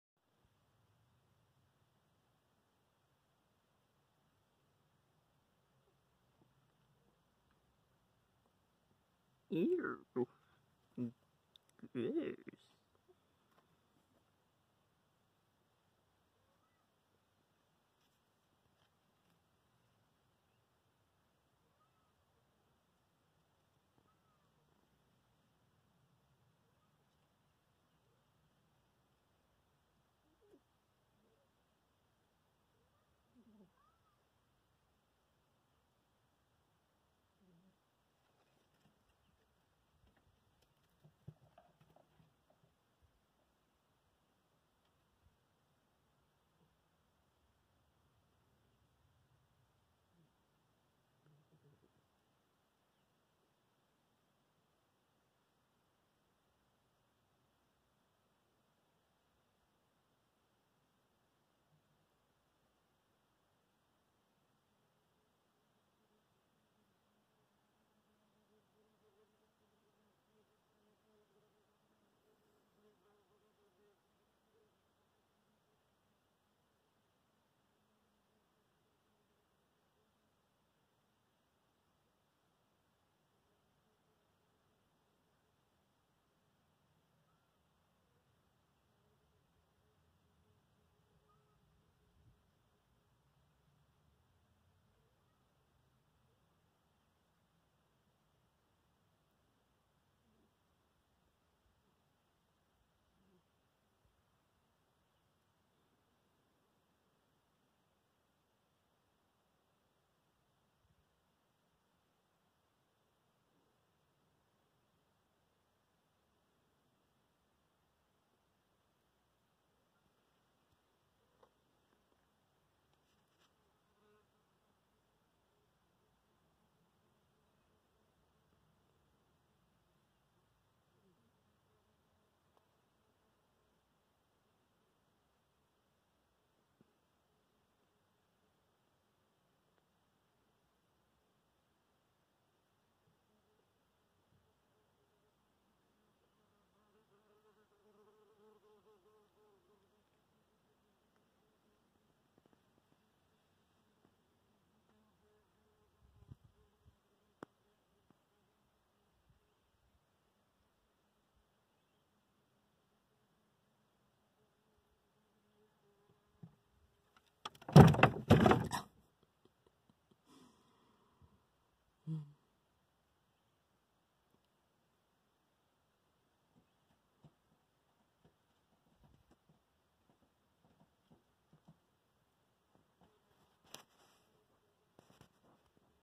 Licorice Slug Eats Dead Mouse.
Slugs eat many things dead. This mouse carcass had been there awhile and some of the skin became exposed, slugs do not like to eat fur. Mostly slugs like fresh veggies and flowers.